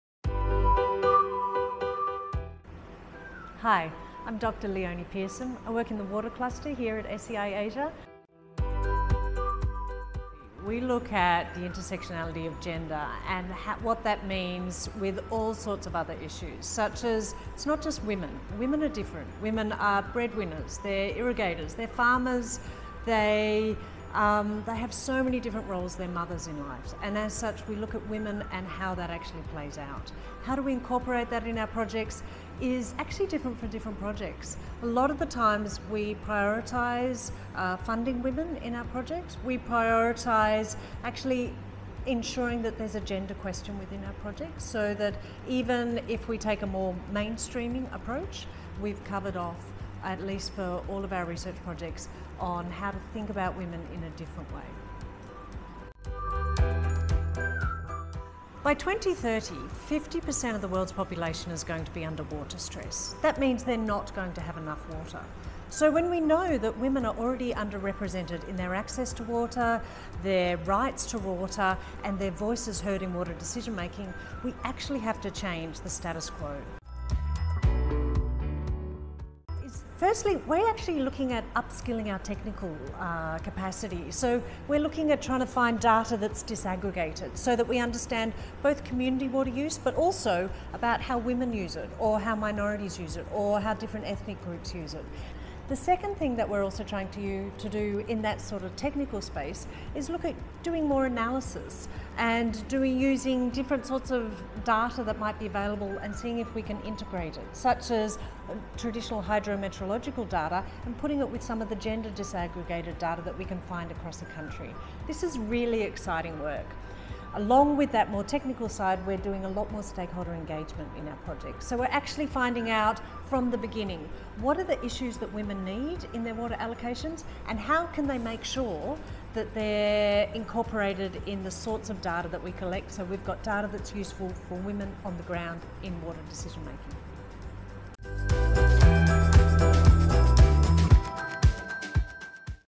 Leonie Pearson, Senior Research Fellow, Water Cluster, SEI Asia
For International Women’s Day 2021, Leonie Pearson of SEI Asia talks about the importance of understanding how women use water in different ways to men in rural and urban settings.